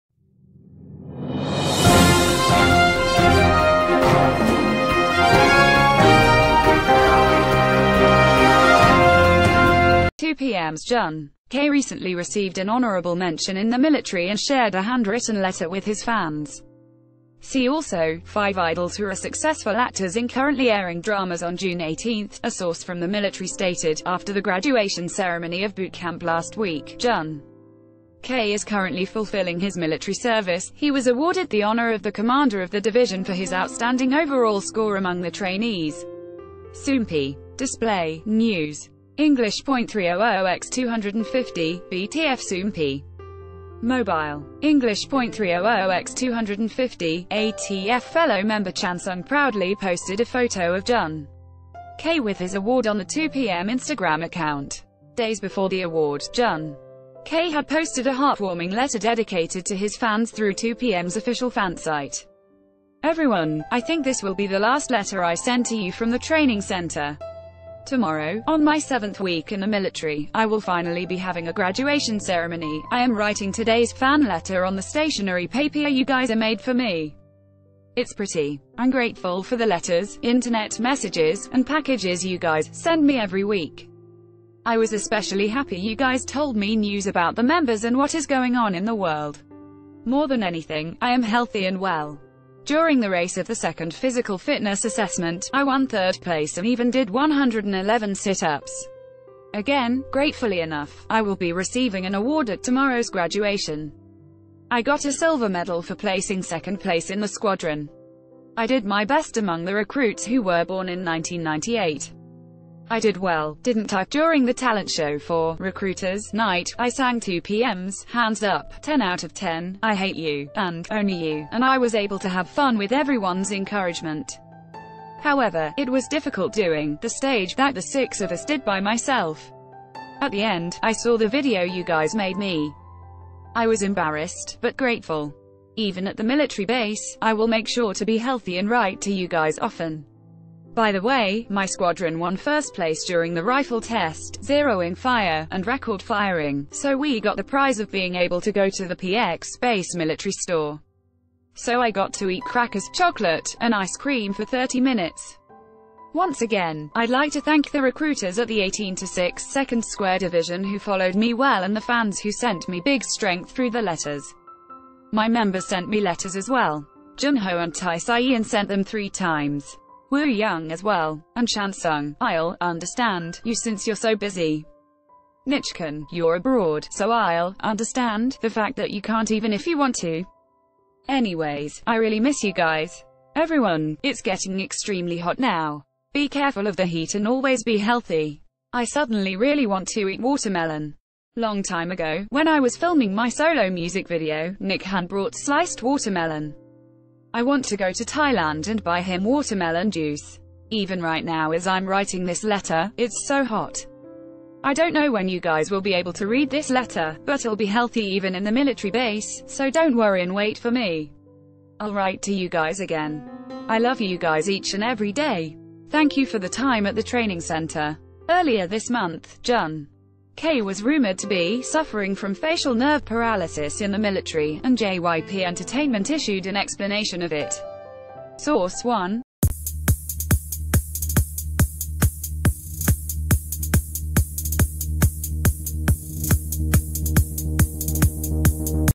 2PM's Jun.K Receives Commendation + Writes Handwritten Letter To Fans From The Army
2PM's Jun.K Receives Commendation + Writes Handwritten Letter To Fans From The Army
2PM’s Jun. K recently received an honorable mention in the military and shared a handwritten letter ...